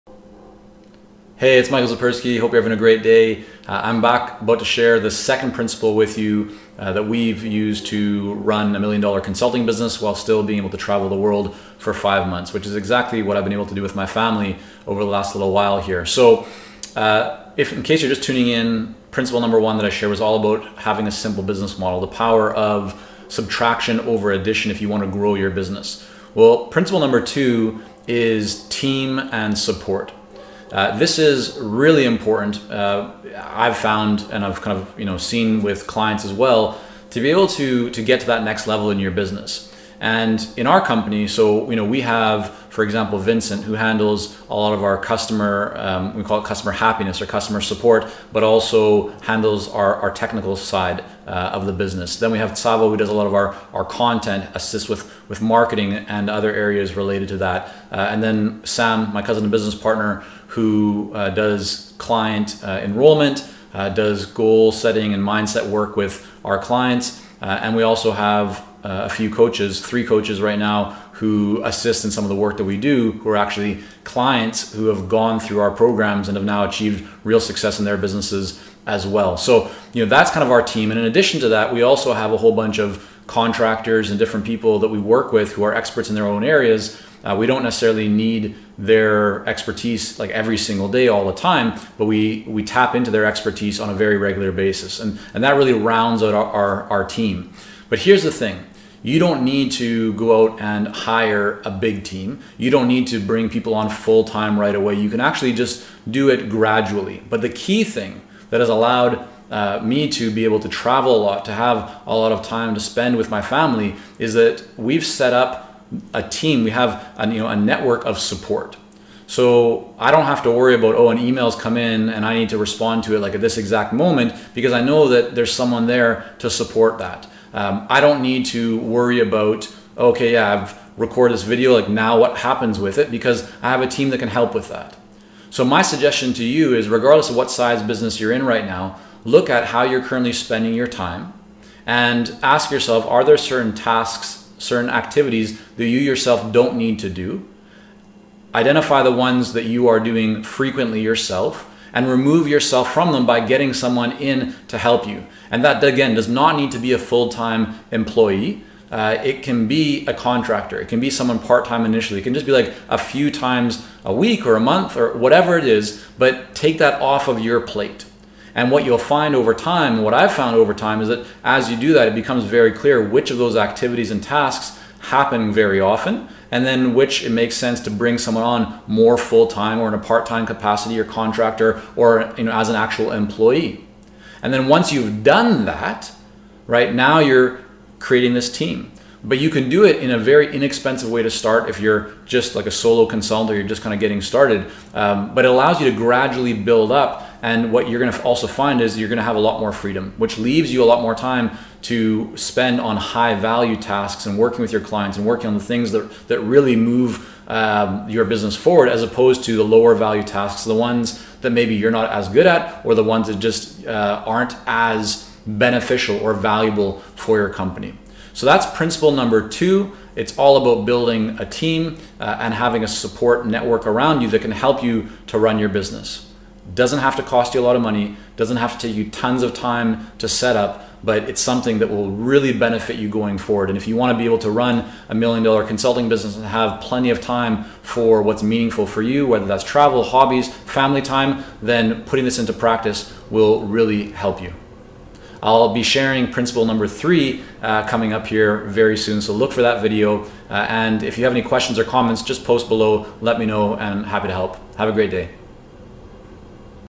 Principle #2 to Run a Million Dollar Consulting Business While Traveling the World
Michael Zipursky of ConsultingSuccess.com shares 6 principles he uses to run a million dollar consulting business while traveling the world with his family.